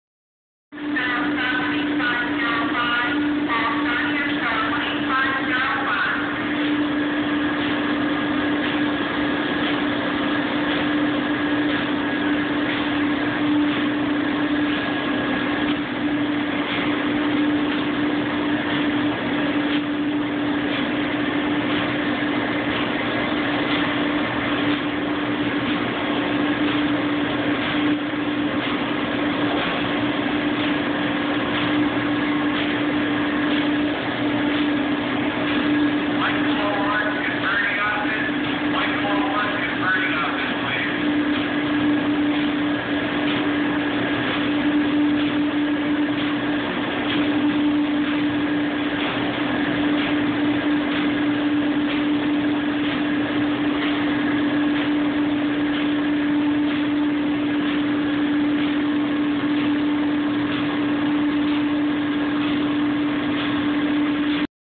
Alar Pilot Study
This is a video of a pilot study being performed at a paper coating company.  The Alar System is a vacuum filtration process that uses diatomaceous earth as a coating for a rotary drum.